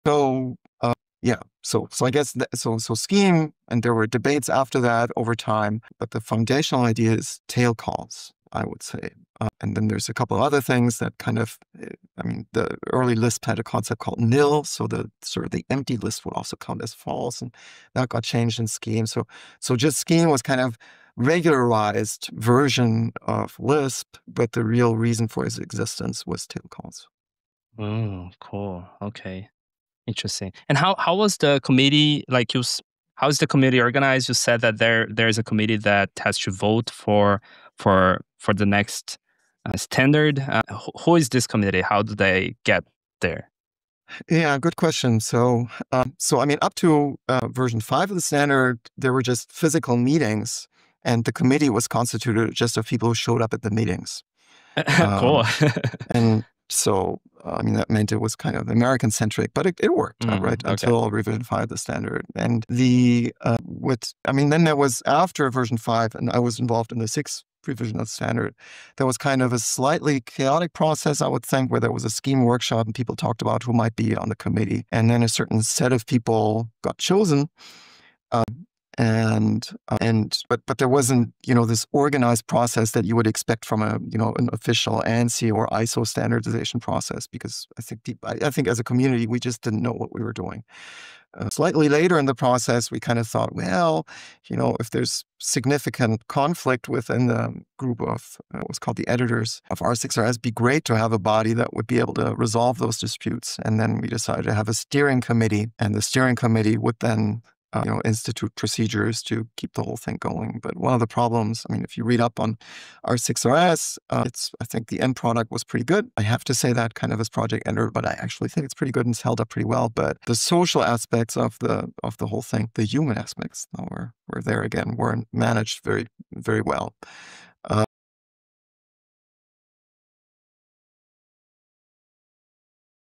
Scheme's Secret: The Chaotic Birth of a Programming Language
Explore the journey of standardizing Scheme, from its essential features like tail calls to the hurdles faced by the standardization committee. A deep dive into language evolution.